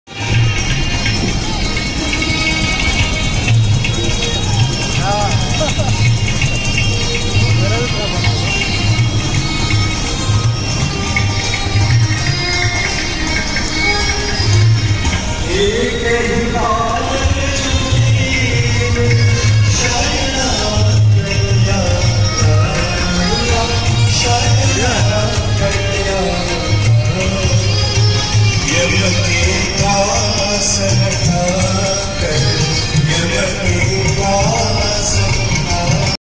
Birthday on Private Steamer Boat with Live Ganga Aarti, Sunset, Ganga View and Family Get-together
Plan a Birthday, small Get-together, Evening Masti on Private Steamer Boat in Patna. we Happen Team will make your moment special and memorable. just plan a Boat ride on Ganga River. we will also doing complete wedding planning services on budget packages.